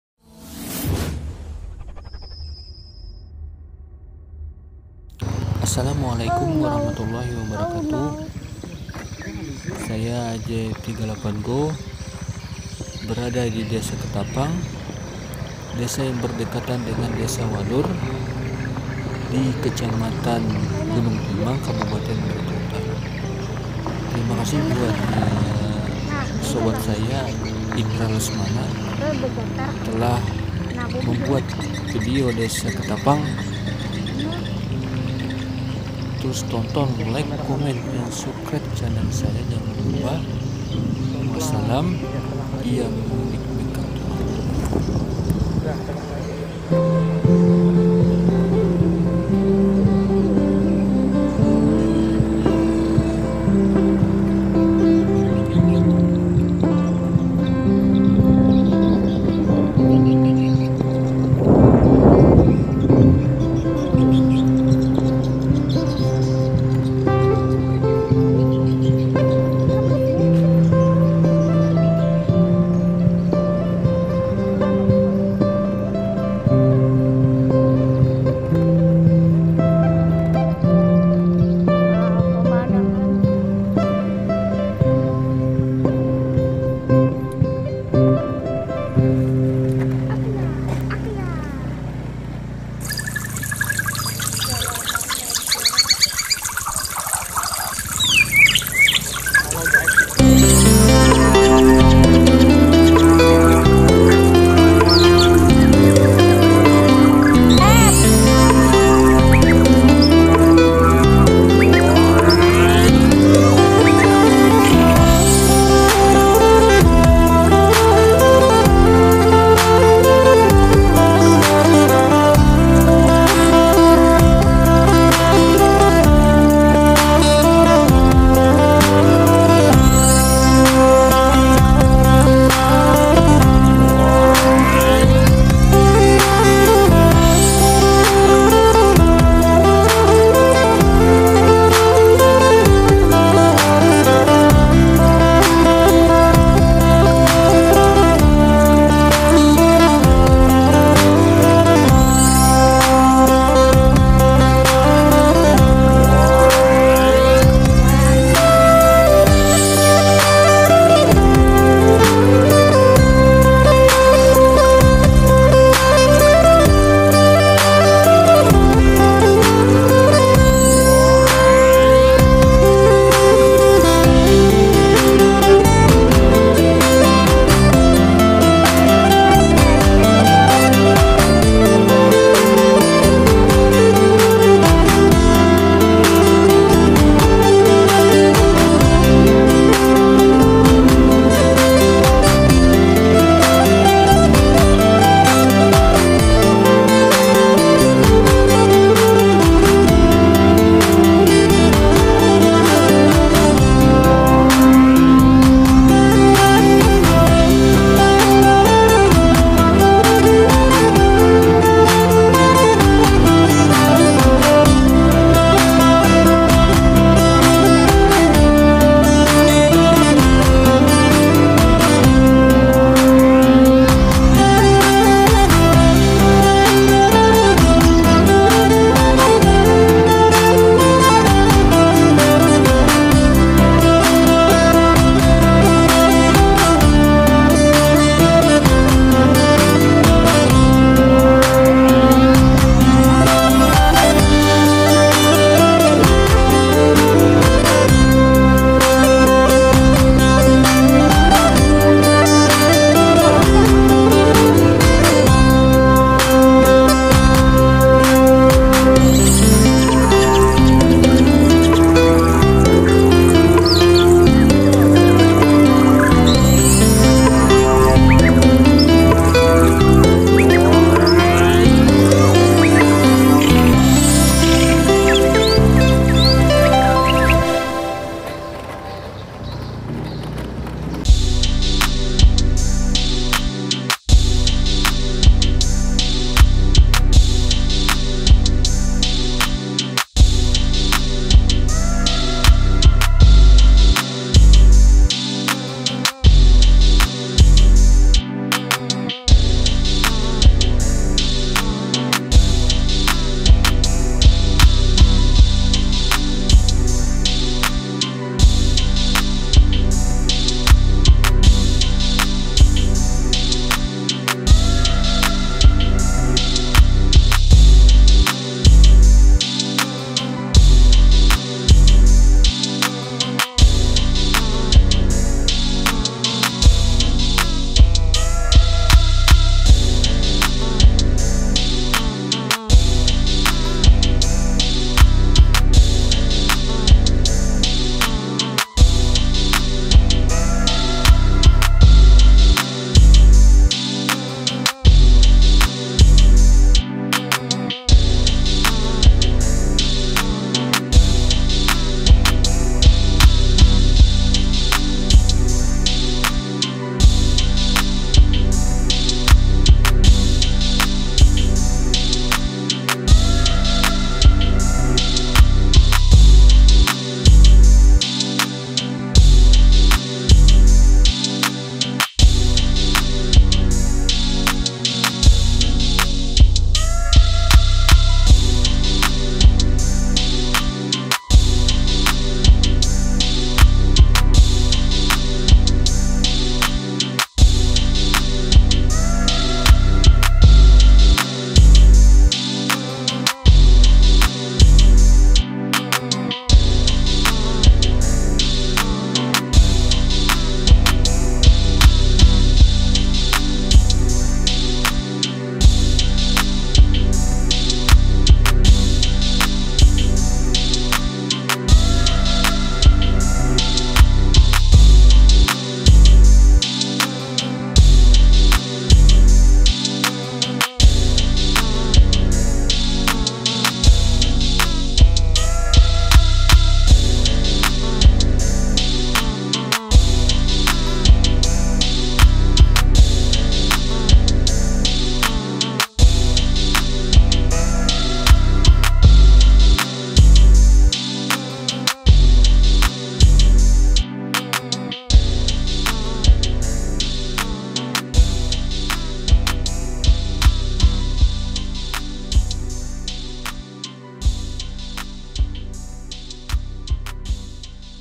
Desa ketapang vlog || gunung timang || barito utara || iphone || Dji Osmo
Ketapang adalah sebuah nama desa di wilayah Gunung Timang, Kabupaten Barito Utara, Provinsi Kalimantan Tengah, Indonesia.

desa ketapang yang di dalam dan diluar jalan raya.

pengambilan video di bulan agustus 2021 menggunakan dji osmo action.

iya mulik bengkang turan.
salam barito utara AJ38 GO